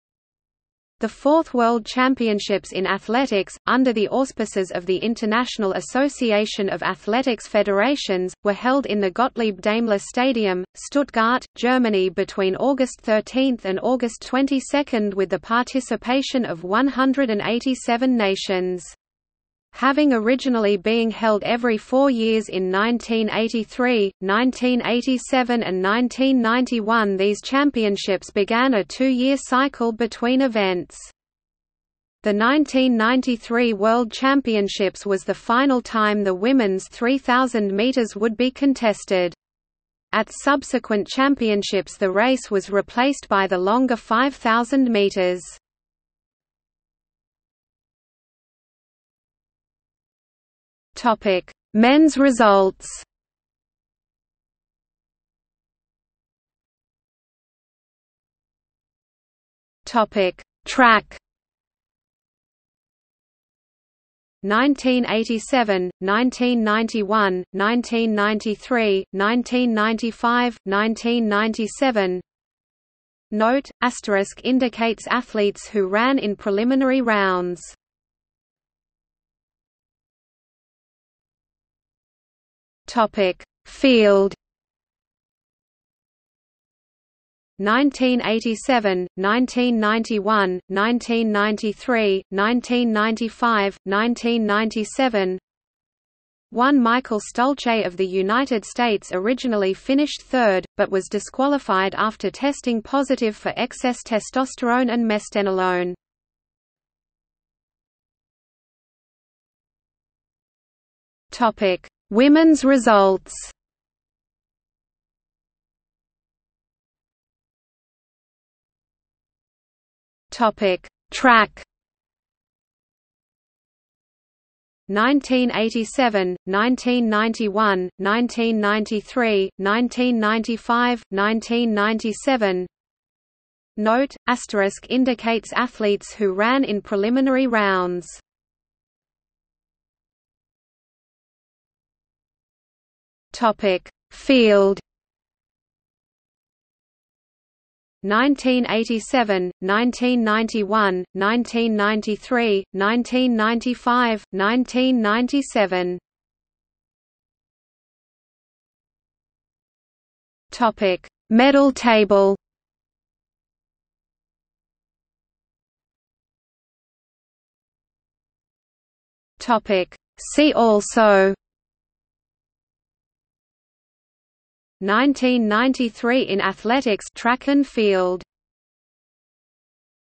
1993 World Championships in Athletics | Wikipedia audio article
00:00:47 1 Men's results
00:00:56 1.1 Track
00:01:19 1.2 Field
00:01:47 2 Women's results
00:01:57 2.1 Track
00:02:20 2.2 Field
00:02:37 3 Medal table
00:02:45 4 See also

- Socrates

SUMMARY
The 4th World Championships in Athletics, under the auspices of the International Association of Athletics Federations, were held in the Gottlieb Daimler Stadium, Stuttgart, Germany between August 13 and August 22 with the participation of 187 nations. Having originally being held every four years in 1983, 1987 and 1991 these championships began a two-year cycle between events.
The 1993 World Championships was the final time the women's 3000 m would be contested. At subsequent Championships the race was replaced by the longer 5000 m.